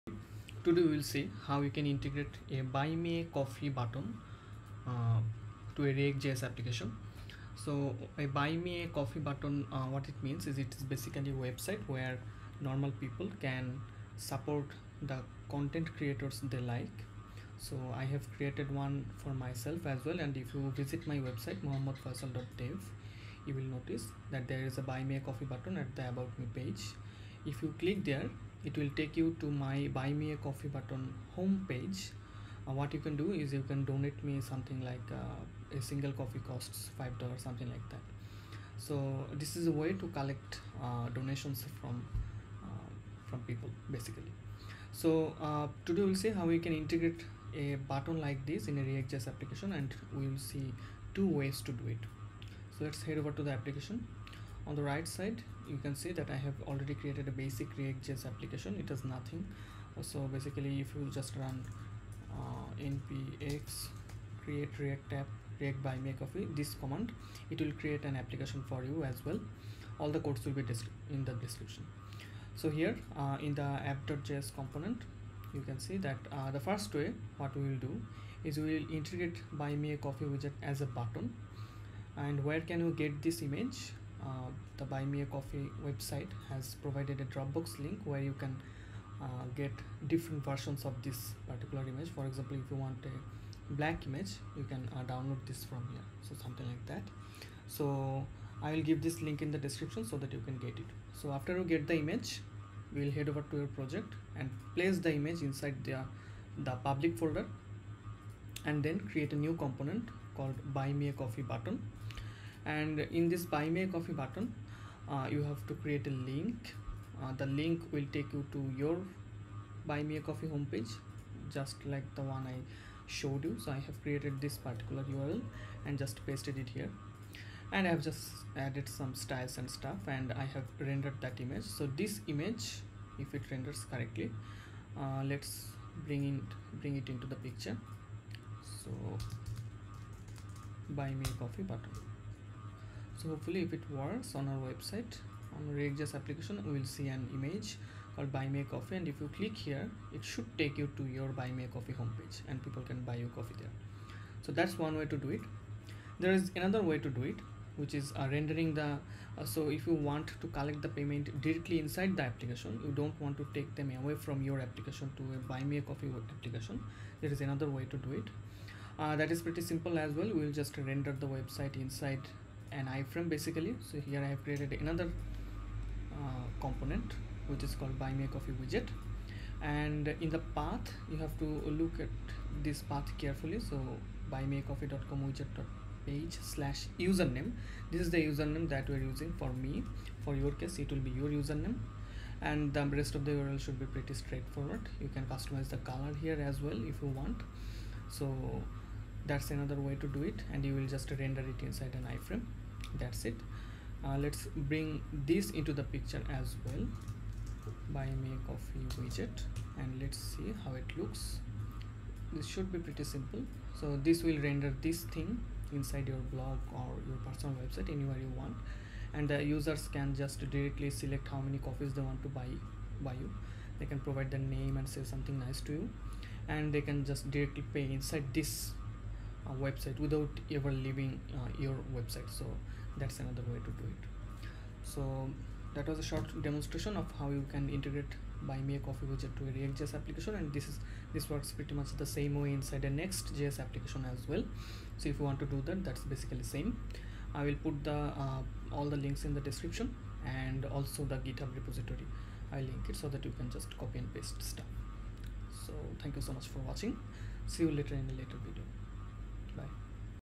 Buy me a coffee button in ReactJS
In today's video, we will see how we can integrate the Buy me a coffee button into a ReactJS application.